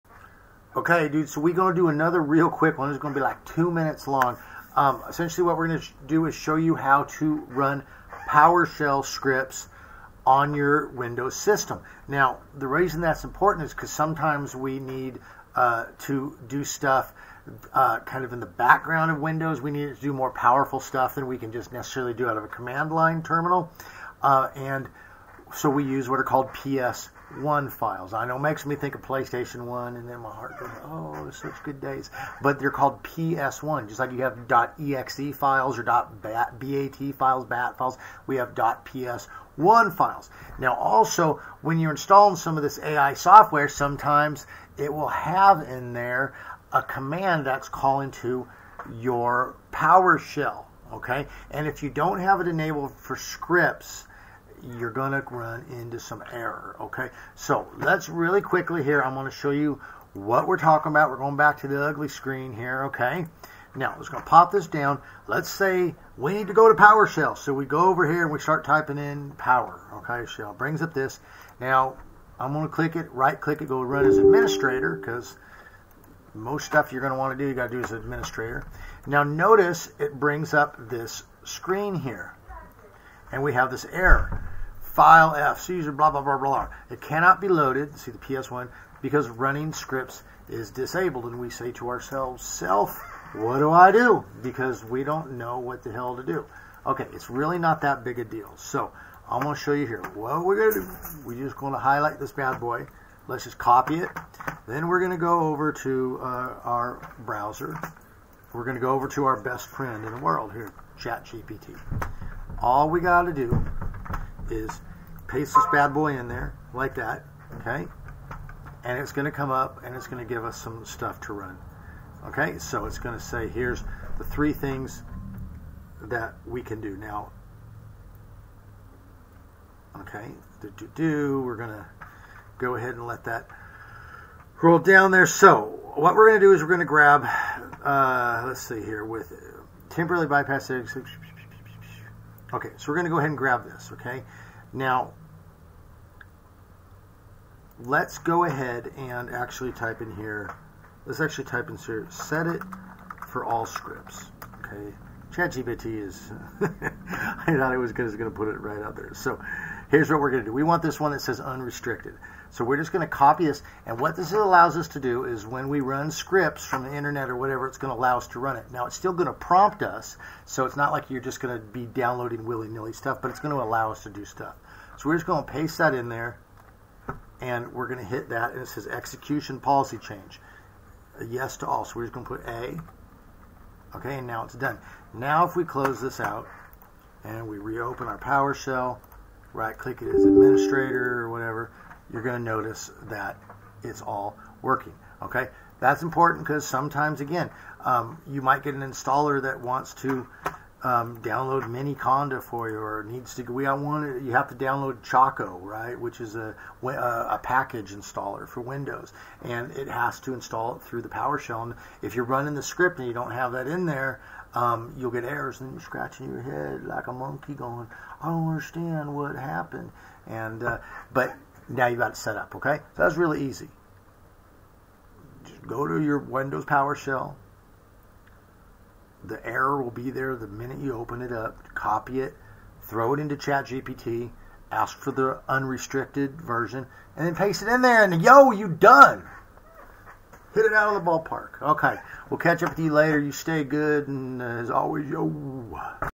Running Powershell Scripts in Windows
In this video, we show you how to quickly enable PowerShell script execution in Windows using the Command Prompt. This is important when installing AI applications that make calls to Powershell (for example, to download Choco).  Follow these simple steps to change the execution policy and start running your scripts without any hassle!